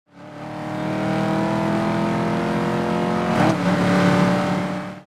Endless Engines Challenge - FINAL RENDER
Hello everyone! I have decided to participate in Clint's Endless Engines Challenge and I just had to use Ethan for it. This is my final submission, made with both Maya and UE5. Hope you guys like it!